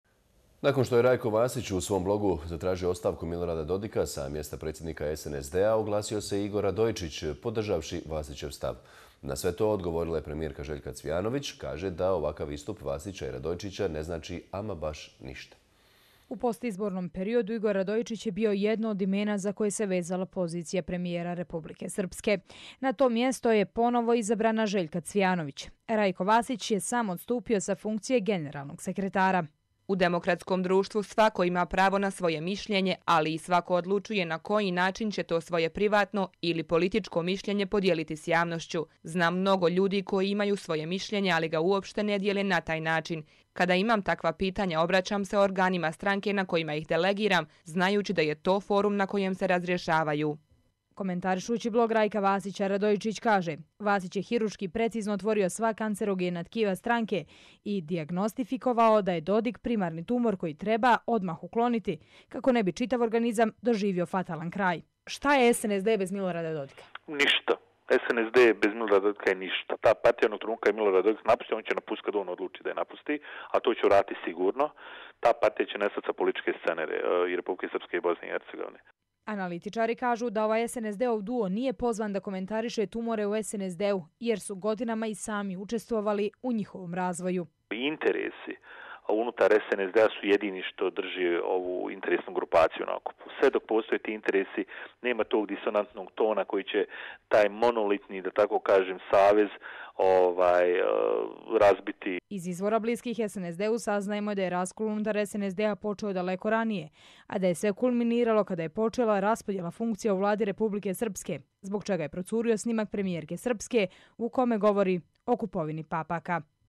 Raskol u SNSD u, nezadovoljstvo Dodikovim vođenjem
Željka Cvijanović, premijer RS: U demokratskom društvu svako ima pravo na svoje mišljenje, ali i svako odlučuje na koji način će to svoje privatno ili političko mišljenje podijeliti s javnošću. Znam mnogo ljudi koji  imaju svoje mišljenje, ali ga uopšte ne dijele na taj način. Kada imam takva pitanja obraćam se organima stranke na kojima ih delegiram, znajući da je to forum na kojem se razrješavaju.

Analitičari kažu da ovaj SNSD-ov duo nije pozvan da komentariše tumore u SNSD-u, jer su i sami godima učestvovali u njegovom razvoju.

Aleksandar Trifunović, politički analitičar: Interesi unutar SNSD-a su jedini što drže ovu interesnu grupaciju na  okupu, sve dok postoje ti interesi nema tog disonantnog tona koji će taj monolitni, da tako kažem savez razbiti ili da tako kažem učiniti slabim.

Komentarišući blog Rajka Vasića Radojičić kaže: Vasić je hirurški precizno otvorio sva kancerogena tkiva stranke i dijagnostikovao da je Dodik primarni tumor. koji treba odmah otkloniti, kako ne bi čitav organizam doživio fatalan kraj.

Aleksandar Trifunović, politički analitičar: Šta je SNSD bez Dodika? Ništa, SNSD bez Milorada Dodika je ništ onog trenutka kada je Milorad Dodika napusti će prestati da postoji. A to će uraditi sigurno. Ta partija će nestati sa političke scene i RS i BiH.

Iz izvora bliskih SNSD-u saznajemo da je raskol unutar SNSD-a počeo daleko ranije, a  da je sve kulminiralo kada je počela raspodjela funkcija u Vladi RS, zbog čega je procurio snimak premijerke Srpske, u kome govori o kupovini papaka.